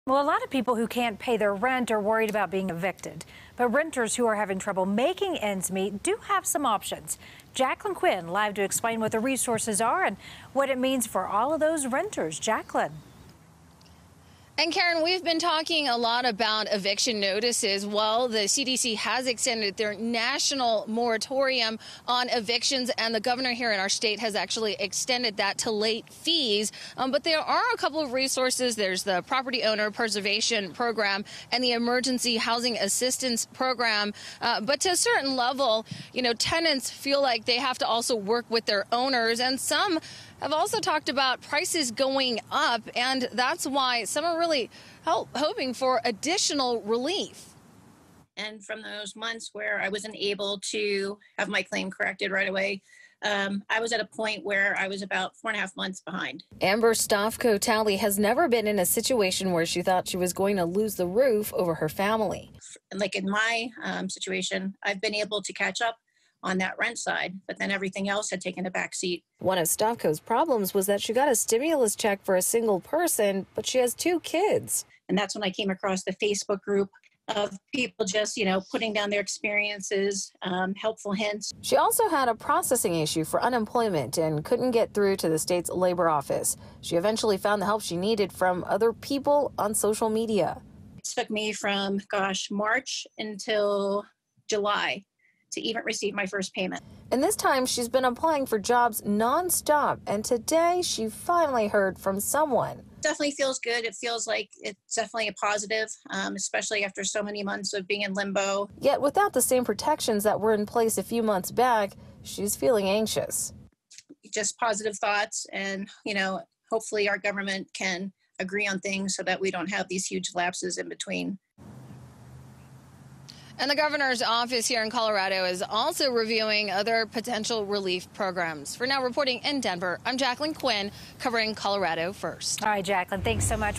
The Eviction Moratorium Wasn't Extended In Colorado But Renters Still Have Options
Those who can't make rent can apply to the Emergency Housing Assistance Program for help during the pandemic.